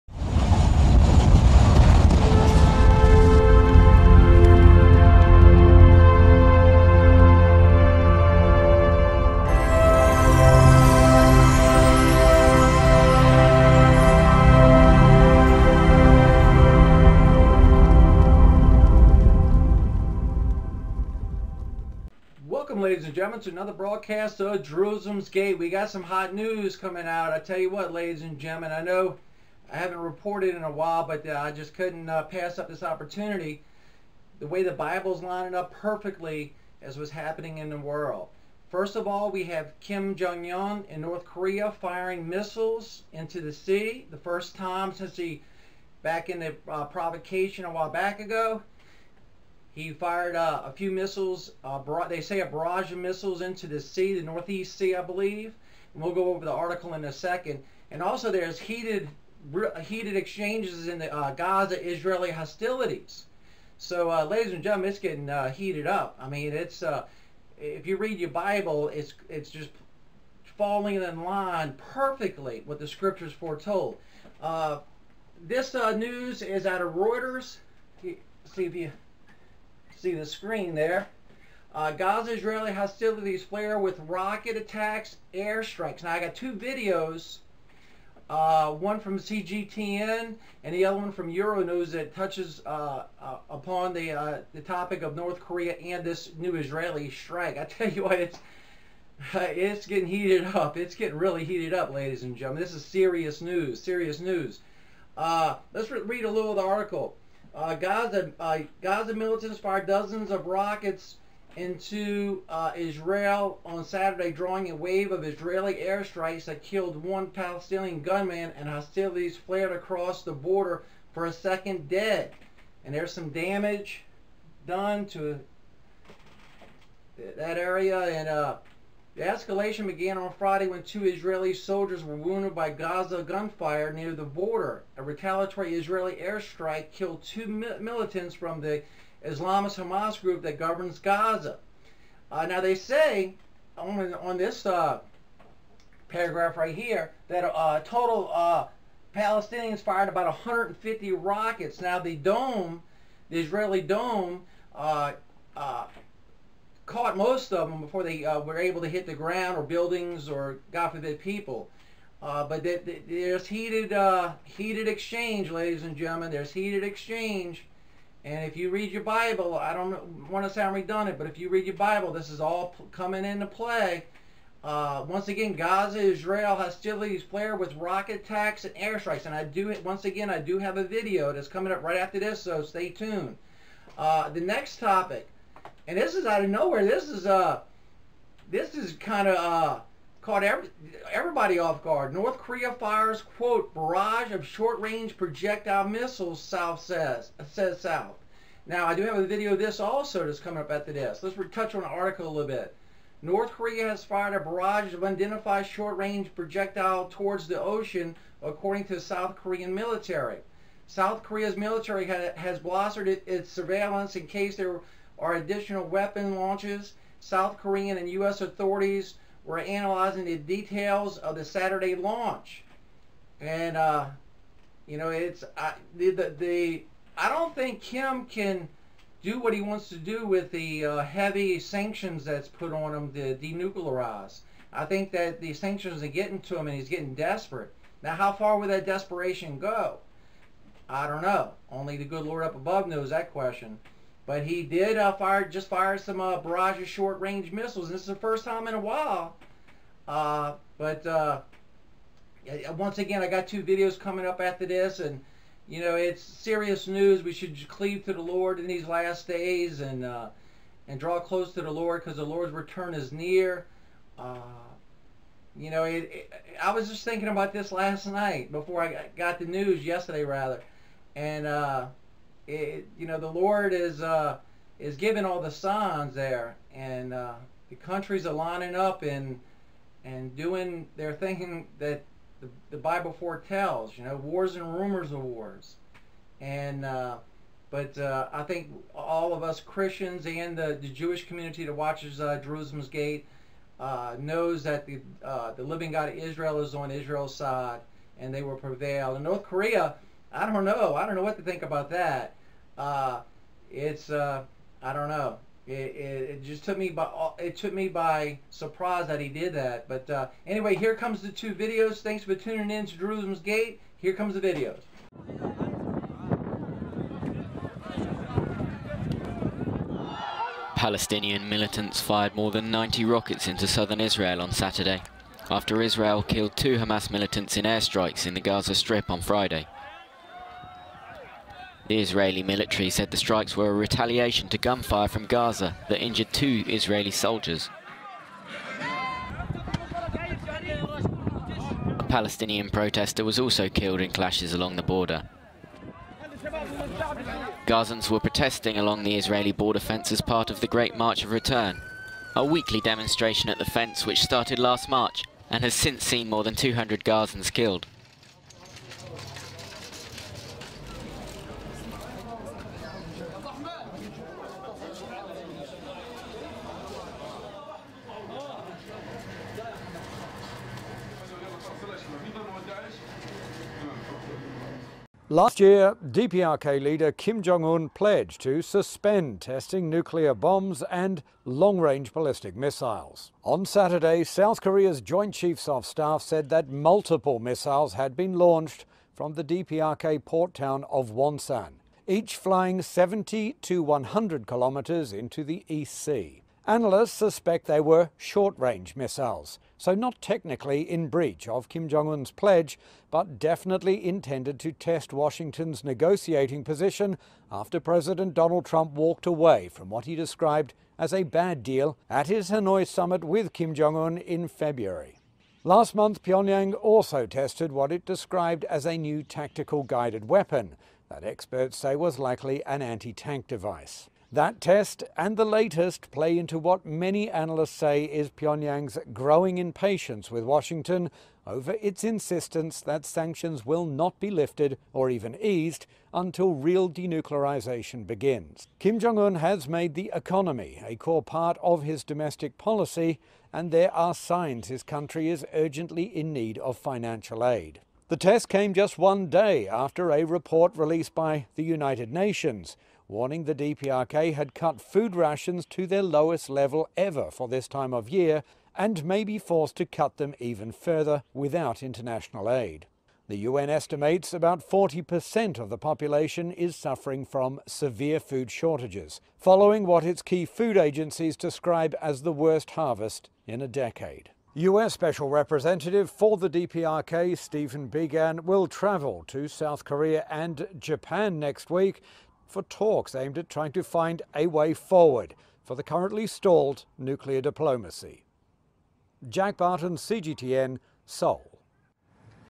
Breaking News! Israel strikes Hamas back! North Korea launch missiles!
Israel and Hamas strikes.  North Korea launches missiles.  News Source:  Reuters and The Canberra Times.  Video Source:  CGTN and EURO News.  Custom Thumbnail:  Google free pictures.  Jerusalem's Gate makes no money from channel.